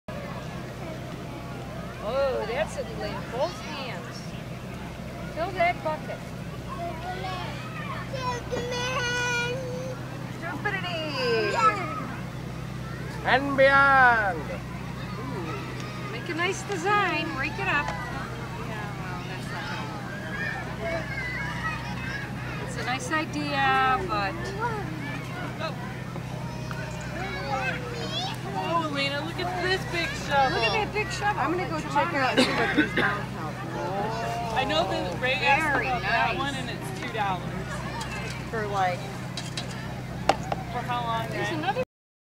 No eating the sand this time
Kiki in the sand at the orchard